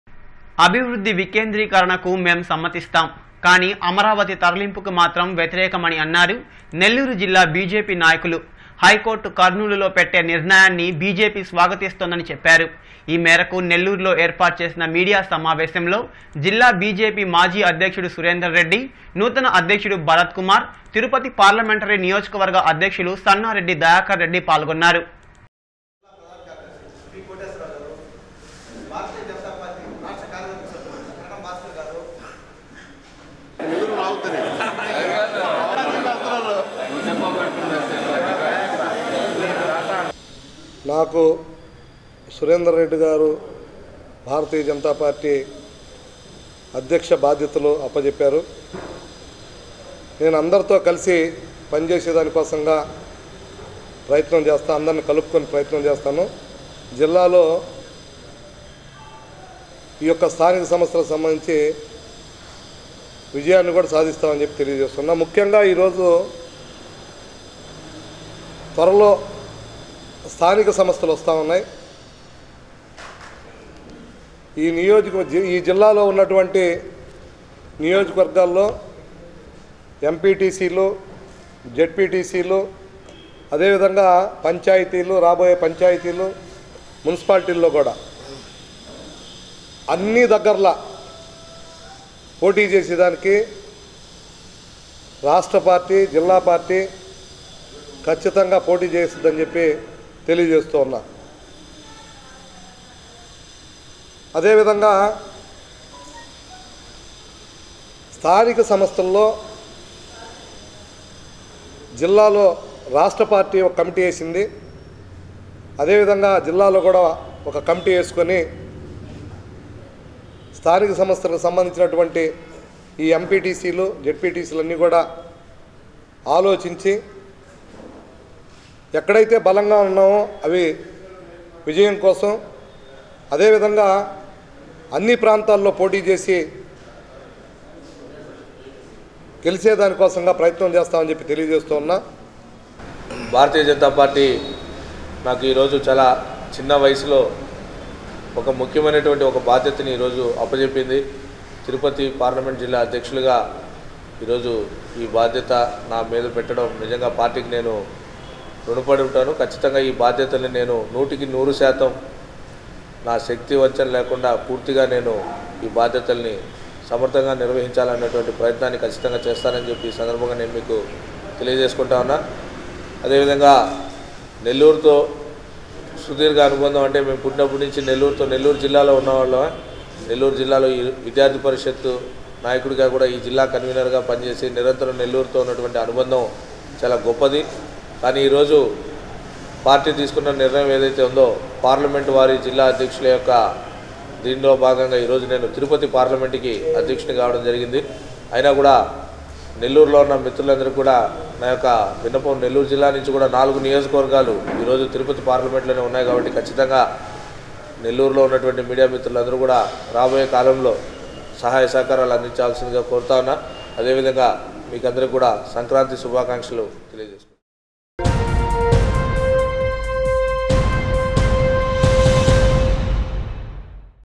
హైకోర్టు కర్నూలులో ఓకే.. రాజధాని తరలిస్తే ఒప్పుకోం.. - NDN News
హైకోర్టు కర్నూలులో ఓకే.. రాజధాని తరలిస్తే ఒప్పుకోం..
అభివృద్ధి వికేంద్రీకరణకు మేం సమ్మతిస్తాం కానీ అమరావతి తరలింపుకి మాత్రం వ్యతిరేకం అని అన్నారు నెల్లూరు జిల్లా బీజేపీ నాయకులు. హైకోర్టు కర్నూలులో పెట్టే నిర్ణయాన్ని బీజేపీ స్వాగతిస్తోందని చెప్పారు. ఈమేరకు నెల్లూరులో ఏర్పాటు చేసిన మీడియా సమావేశంలో జిల్లా బీజేపీ మాజీ అధ్యక్షులు సురేంద్ర రెడ్డి, నూతన అధ్యక్షులు భరత్ కుమార్, తిరుపతి పార్లమెంటరీ నియోజకవర్గ అధ్యక్షులు సన్నారెడ్డి దయాకర్ రెడ్డి పాల్గొన్నారు.